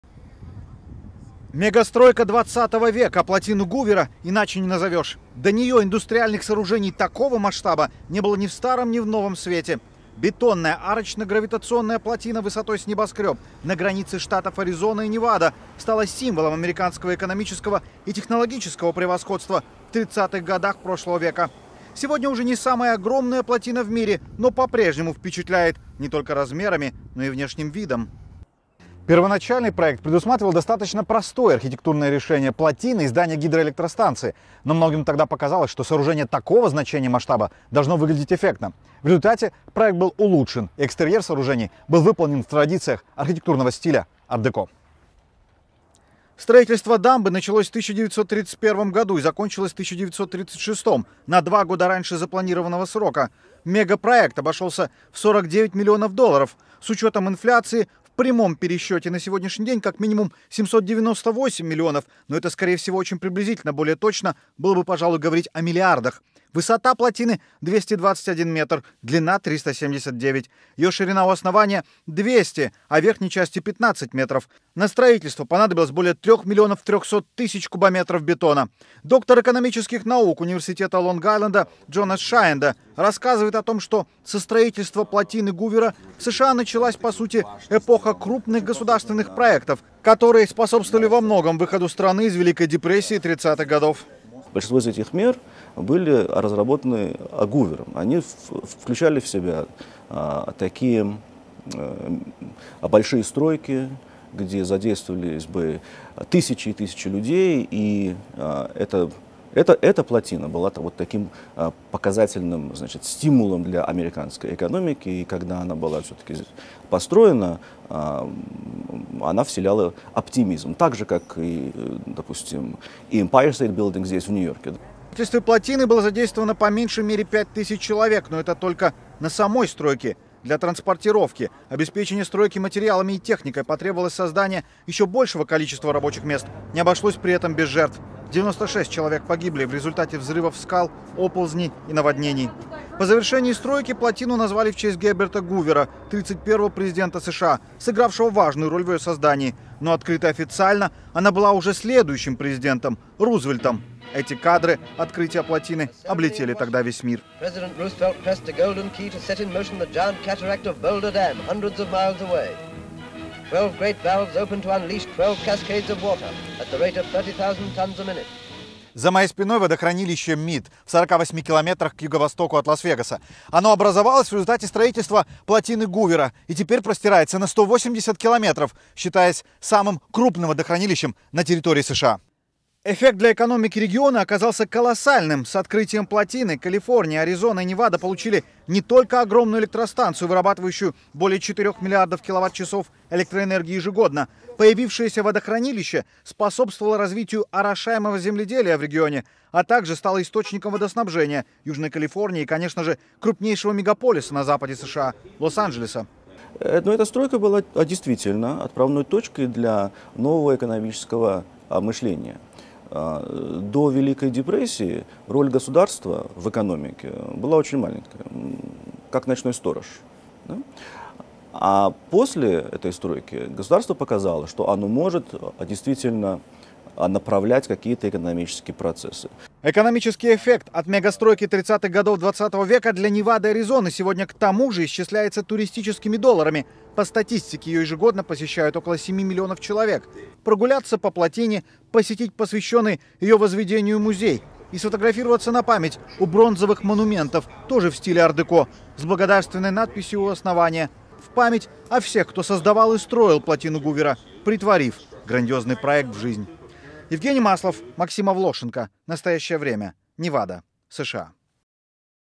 Почему американцы гордятся плотиной Гувера
Строительный мега-проект, преобразовавший юго-запад США